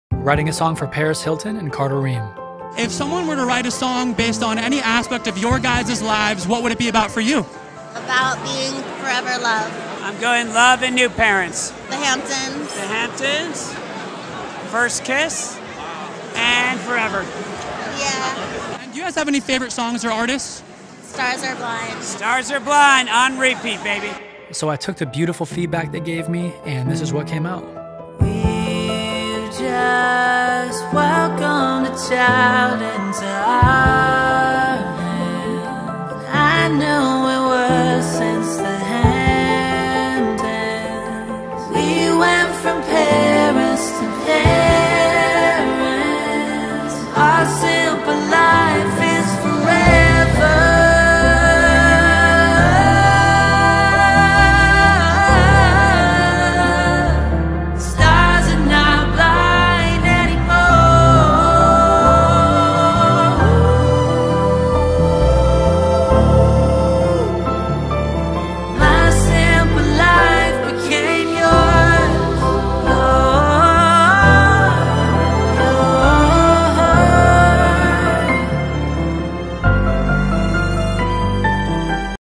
This stranger writes a song for Paris Hilton!
KaleSalad is the number one meme account on the internet!